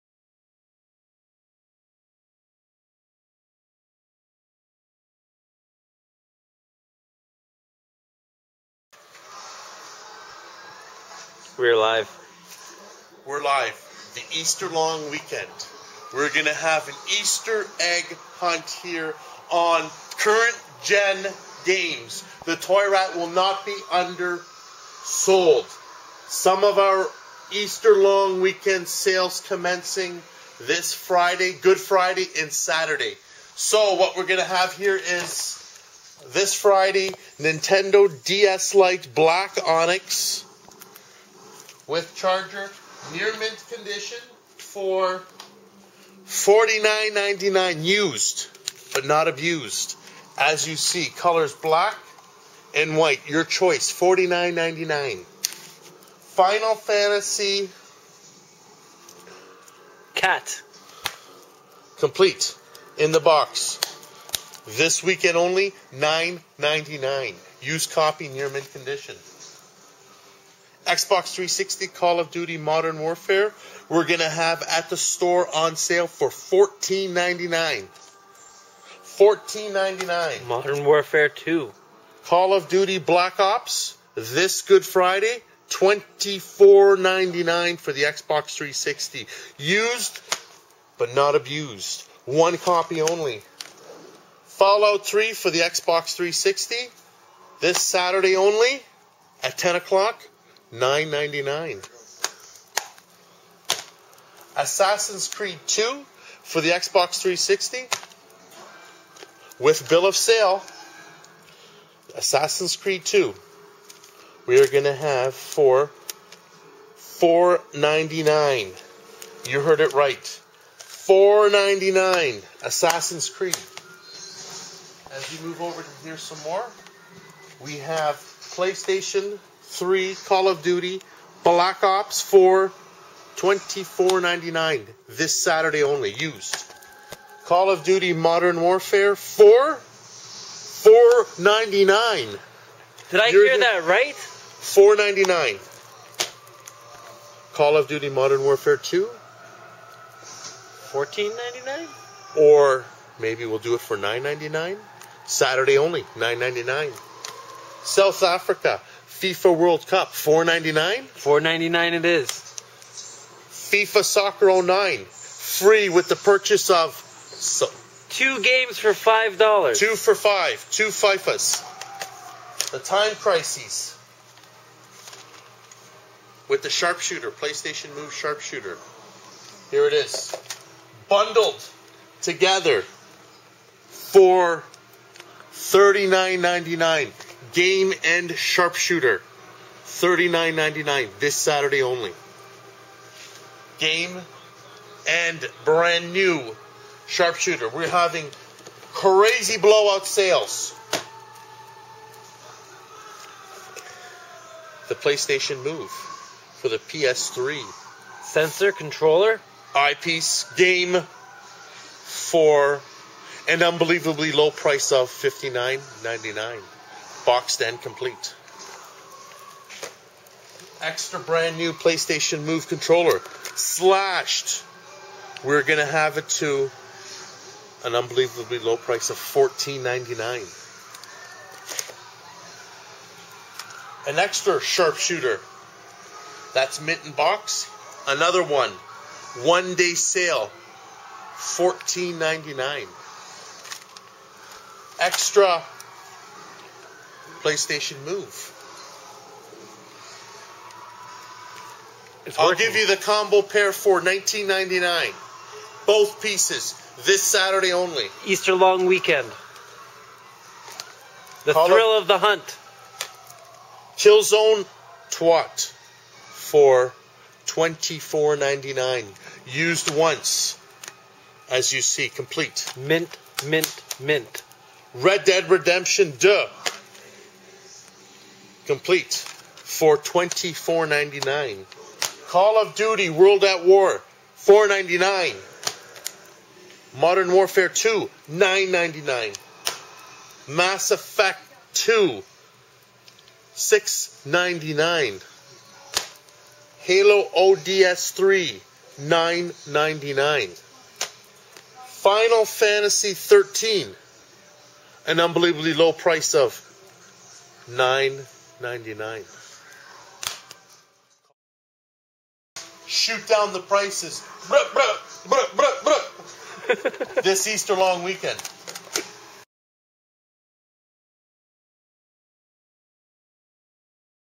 Easter Long Weekend 2011 PS3 + 360 BLOWOUT SALE
We have a very special treat for our customers this Easter Long Weekend - we have just secured a current-gen gaming collection containing key titles.. and we are BLOWING THEM OUT AT ROCK BOTTOM PRICES. You will not find these games cheaper anywhere else in North America, guaranteed.